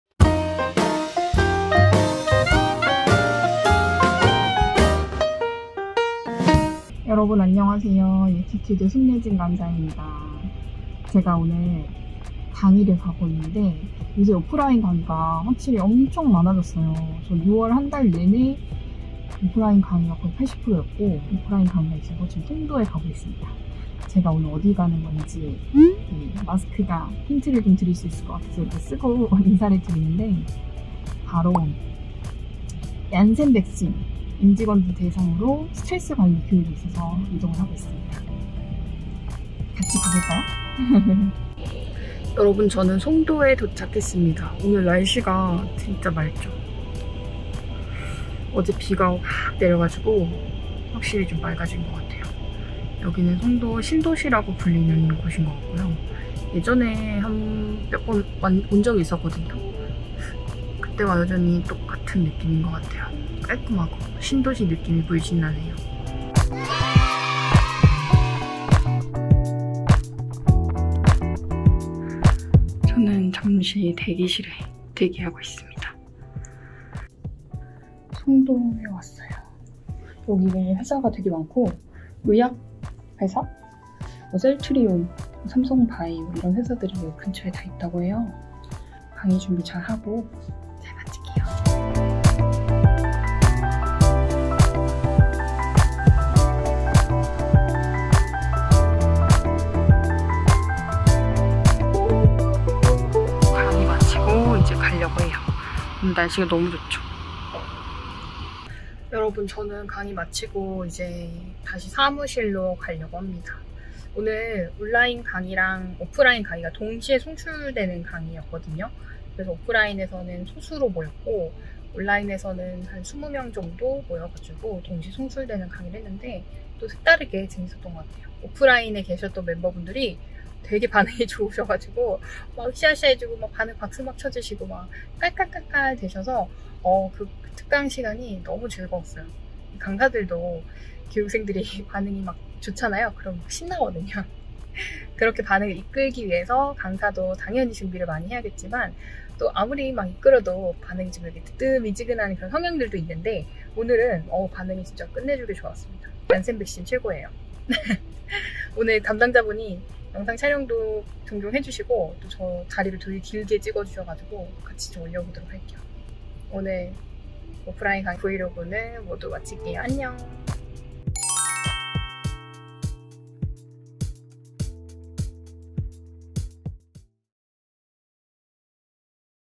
얀센백신 직원들 만나서 강의하고 온 후기(강사브이로그)
얀센백신 직원들 앞에서 강의하고 온 후기
강사 브이로그 영상을 남겨봅니다 :)
영상 보시고 궁금한 점 있으시면
아래 댓글로 문의주세요!^^